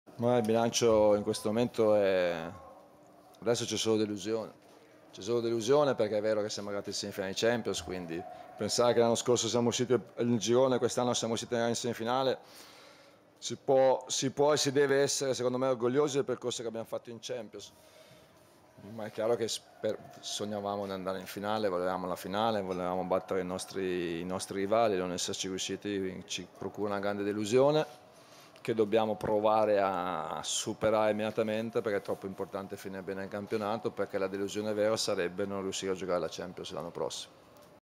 Pioli: "C'è solo delusione"
Pioli amaro: "In questo momento c'è solo delusione, sognavamo la finale" 🔴⚫️